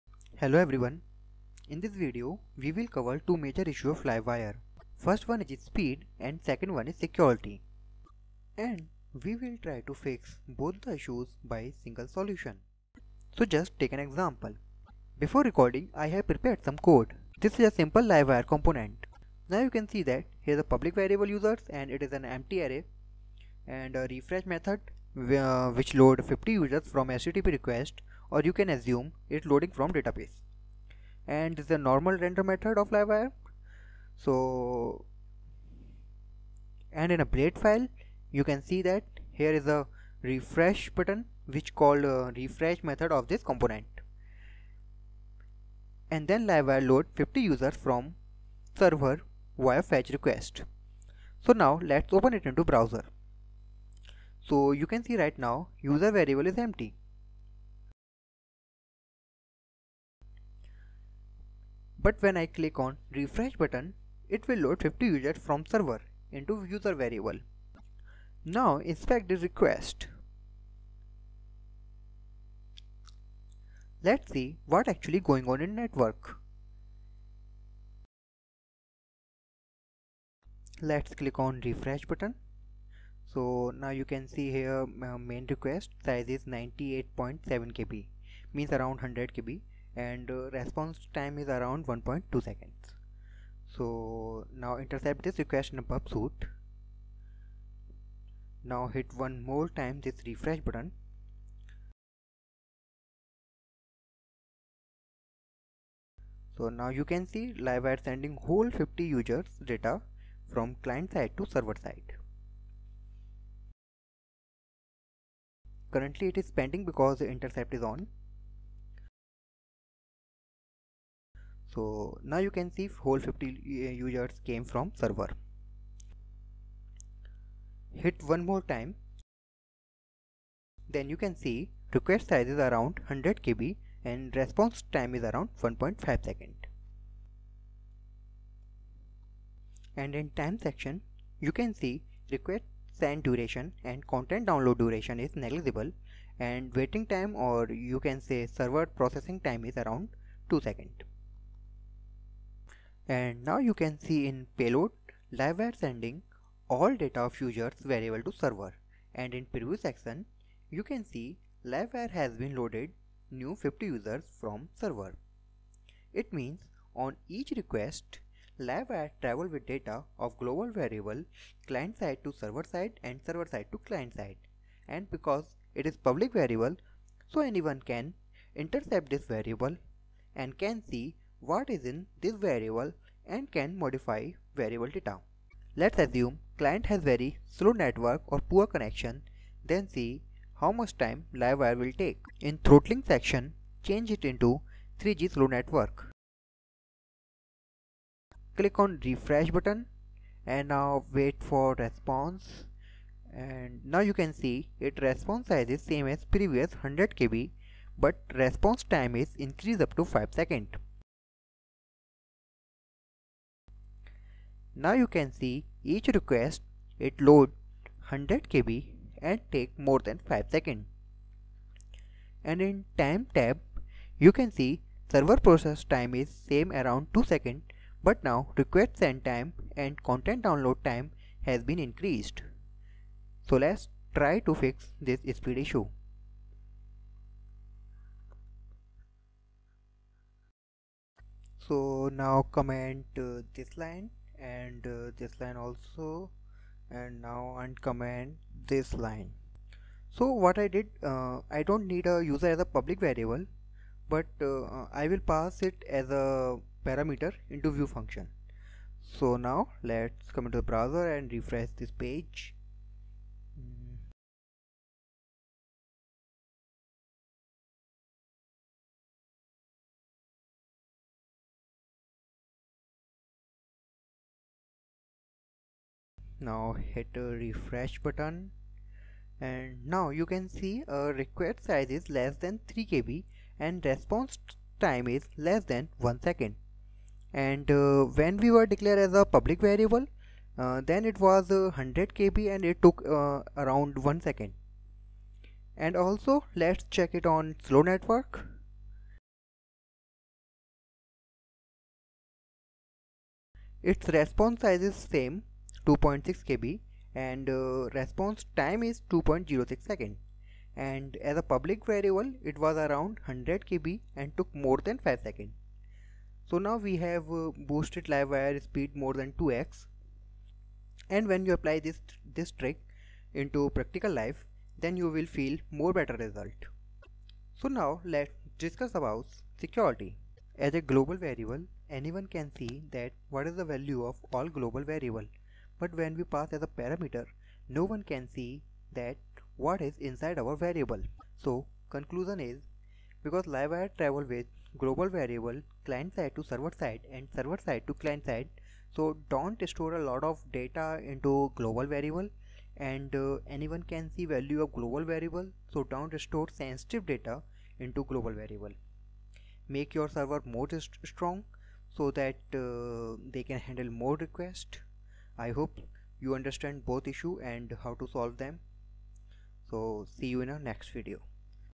Optimizing and Securing Livewire | Faster and Secure Livewire | Speed up Livewire
Let's discuss that how to optimize or speed up Livewire and how to secure your variable in Livewire.

00:00 - Into
00:15 - Code Structure
01:13 - Speed Issue
03:06 - Speed Issue on Slow Network
05:27 - Security Issue